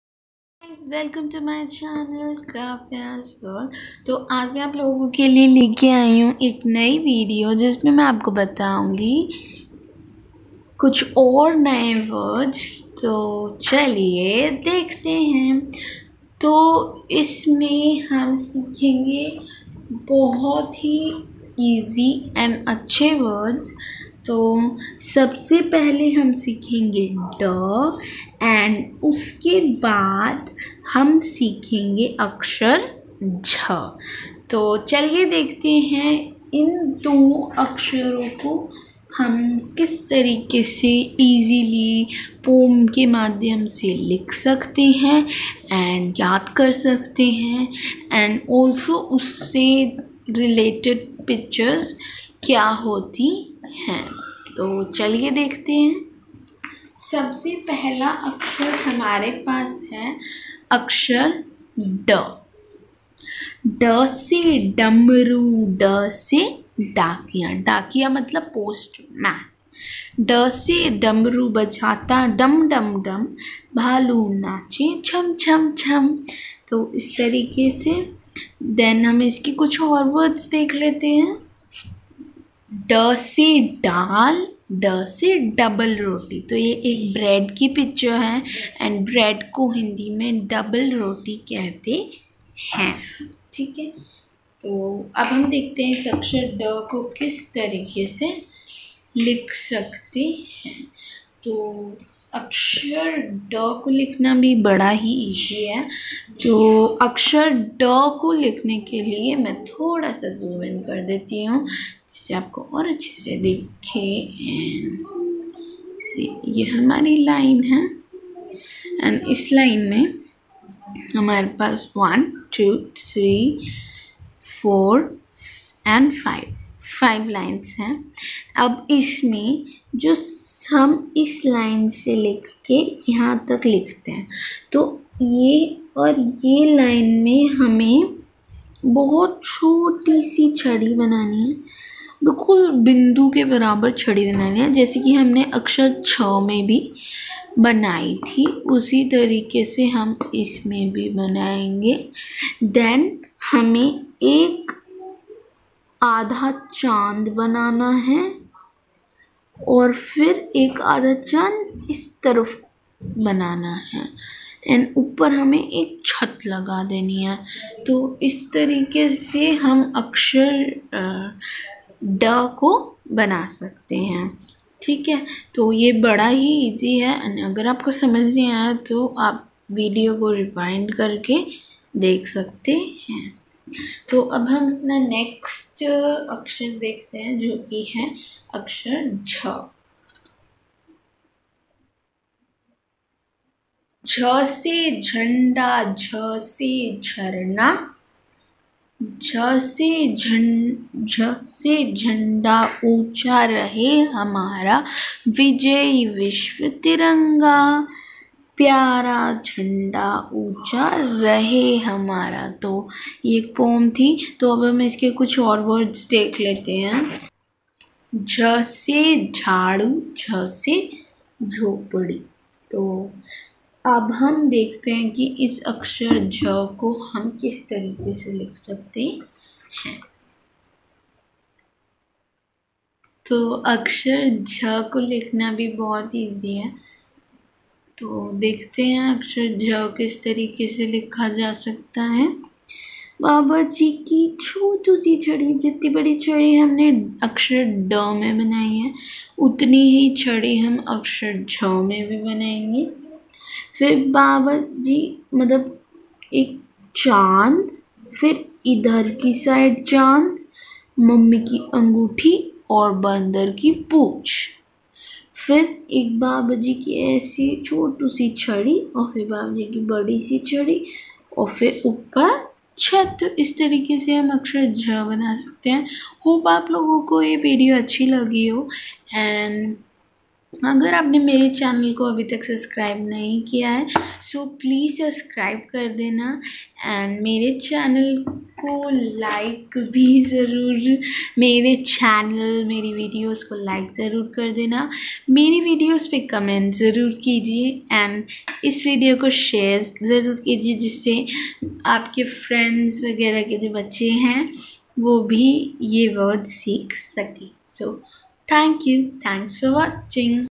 बच्चों को हिंदी घर से कैसे पढाये || Part 7 ||ड और झ || Kavya's World
hope you all liked the vedio

INTRODUCTION : (0:00)
अक्षर ड : (0:56)
अक्षर झ : (3:05)
THANKYOU : (4:35)

kavyagoel3